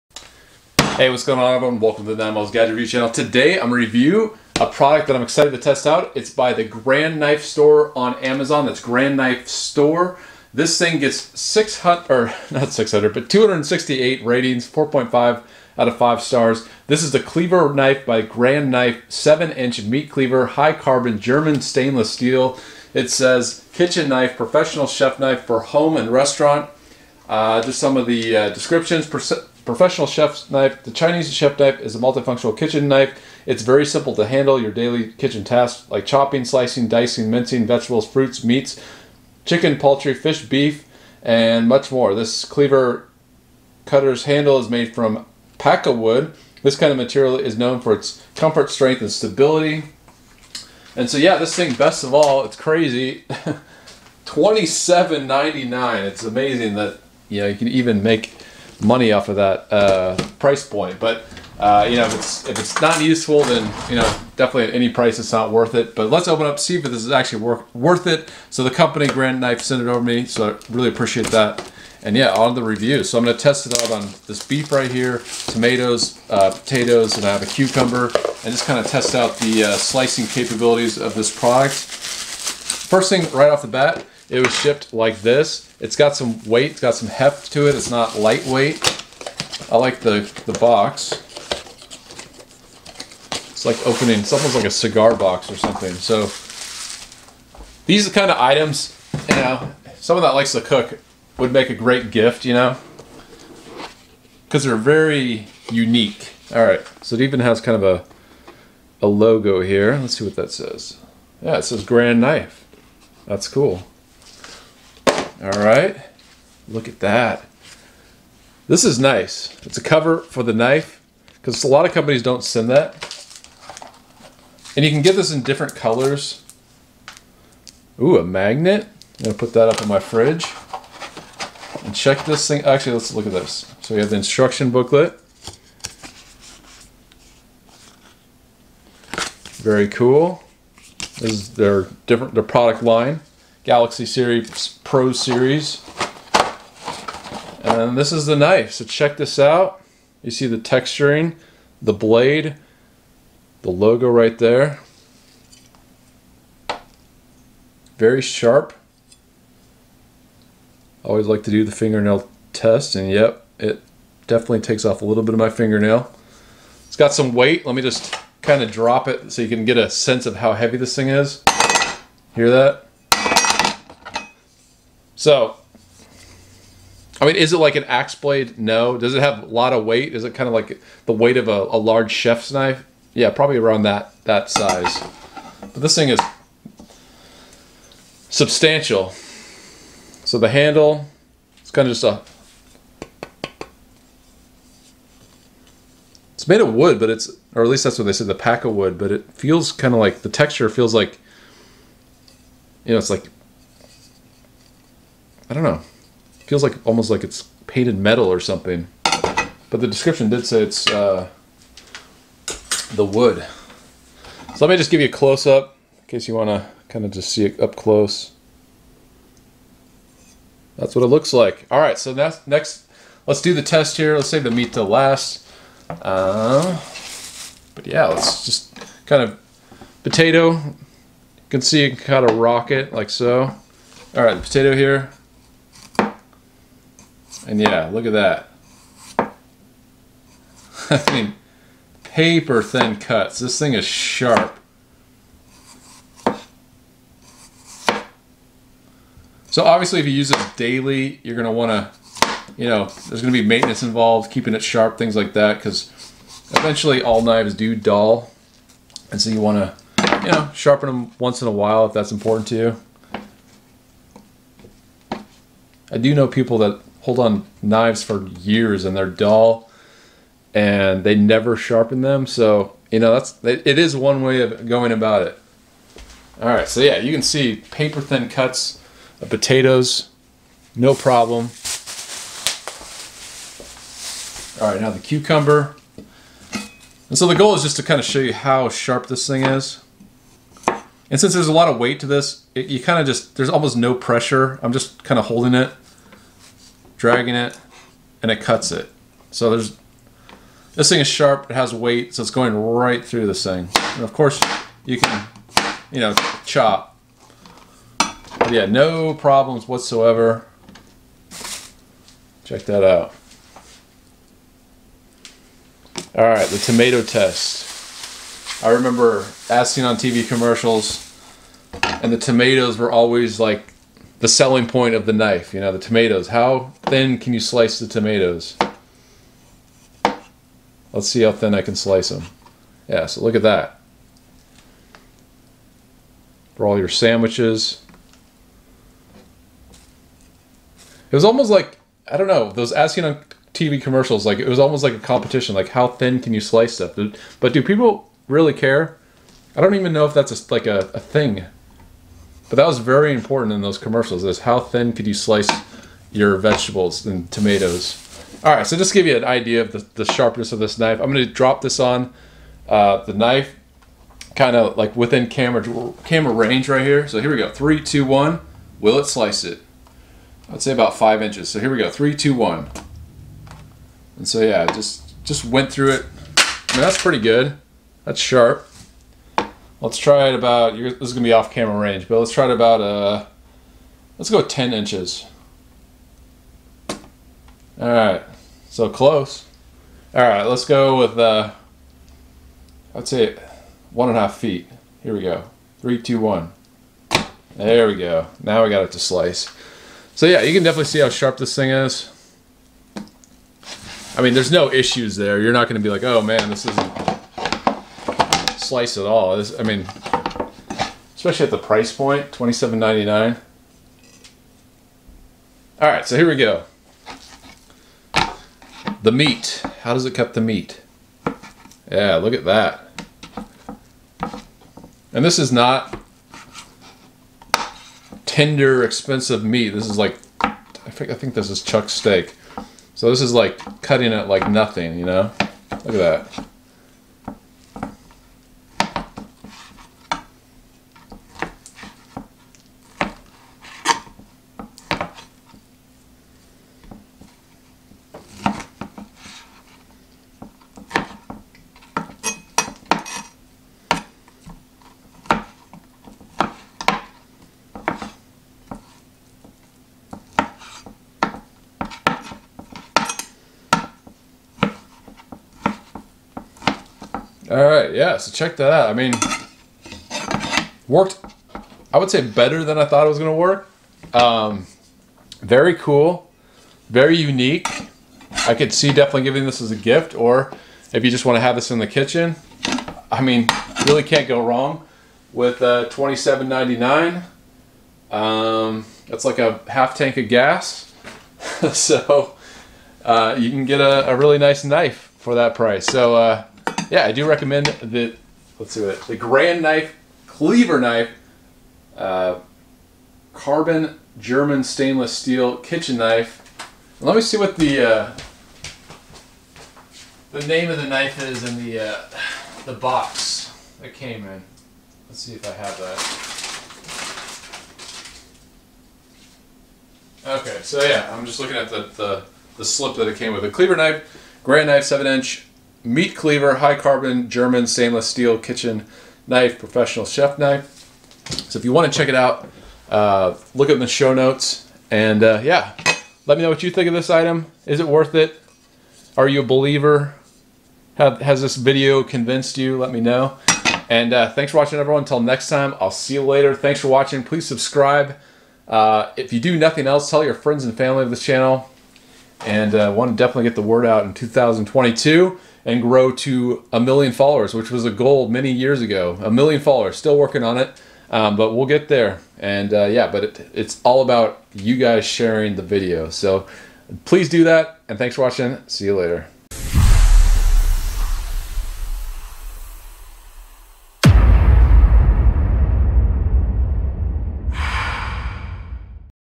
GrandKnife Cleaver 7 Inch High Carbon Stainless Steel Kitchen Knife Review
Watch the 9malls review of the GrandKnife Cleaver 7 Inch High Carbon Stainless Steel Kitchen Knife. Is this Unique Cleaver Knife With Pakkawood Handle actually worth getting? Watch the hands on test to find out.

I test each product on site thoroughly and give high marks to only the best. In the above video I received a free product sample to test. We are independently owned and the opinions expressed here are our own.